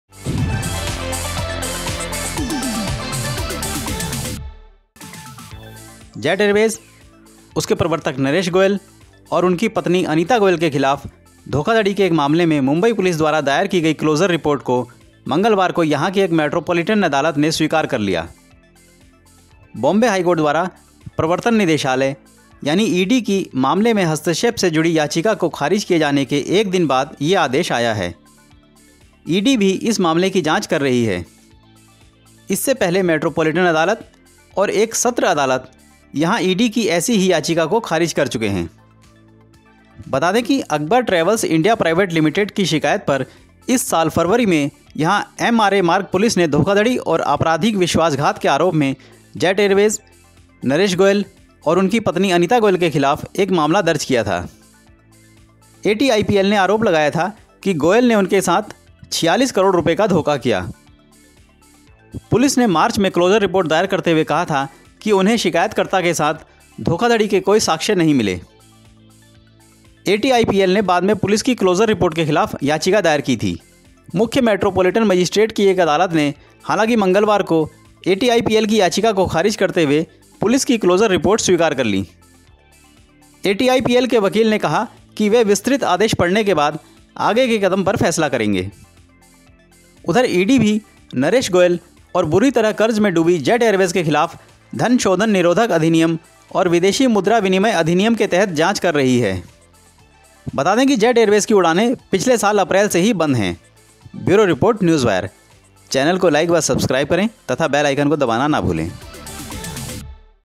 Jet Airways | Naresh Goyal | Jet Airways Latest News | Jet Airways Update | News Wire
Jet Airways Latest News…
MUMBAI: A metropolitan court here on Tuesday accepted the closure report filed by Mumbai police in a cheating case against Jet Airways, which has been grounded since April last year, its promoter Naresh Goyal and his wife. The order came a day after the Bombay High Court rejected a plea of the Enforcement Directorate (ED), which is also probing the case, to intervene into the matter. Earlier, a similar plea by the ED had been rejected by the metropolitan court and a sessions court here…